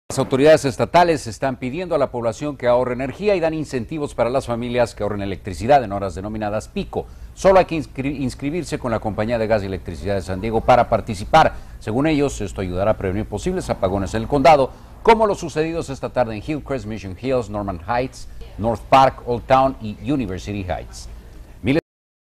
Hay incentivos para ahorrar energía
San Diego -- Autoridades estatales siguen pidiendo a la población que ahorre energía y dan incentivos para las familias que ahorren electricidad en horas denominadas pico, solo hay que inscribirse con la compañía de Gas y Electricidad de San Diego para participar.

Según ellos esto ayudara a prevenir posibles apagones en el condado, como los sucedidos esta tarde en Hillcrest, Mission Hills, Normal Heights, North Park, Old Town y University Heights.